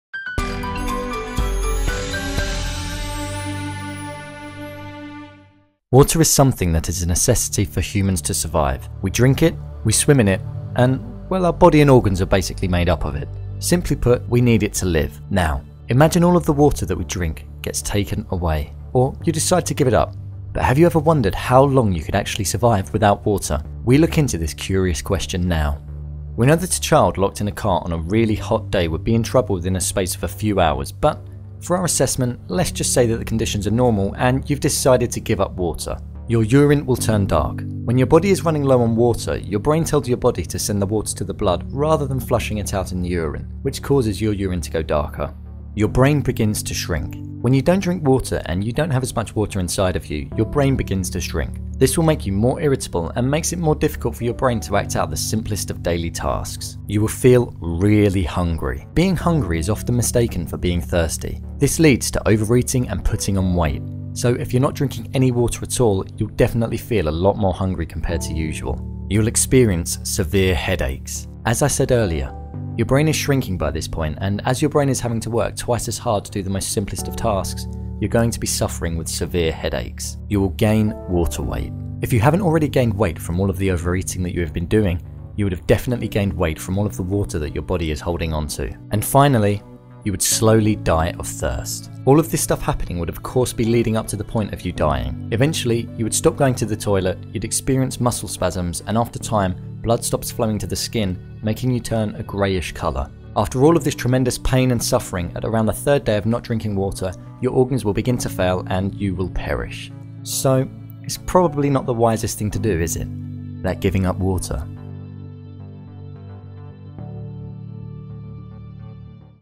What Would Happen If You Stopped Drinking Water?
Water is something that is vital for life to survive. So what would happen if you were to give it up? This video will tell you why.